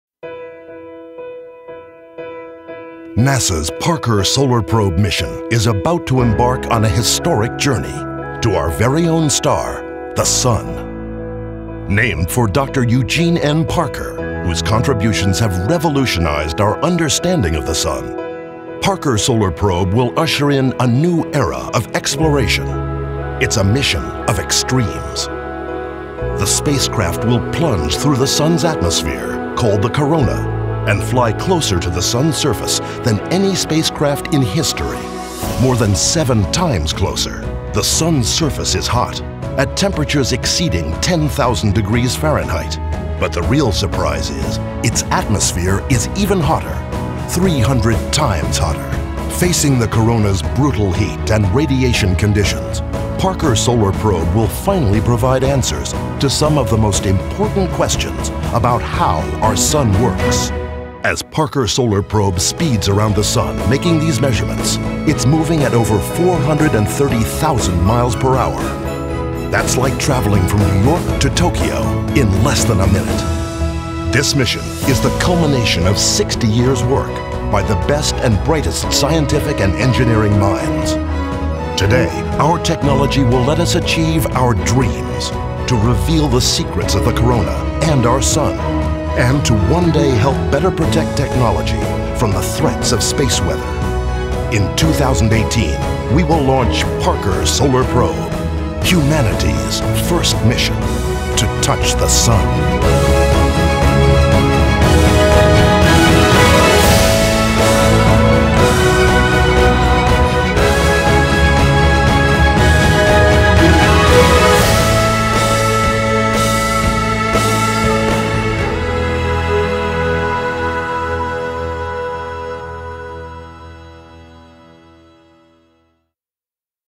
Parker Solar Probe
NASA’s Parker Solar Probe will be the first-ever mission to "touch" the sun.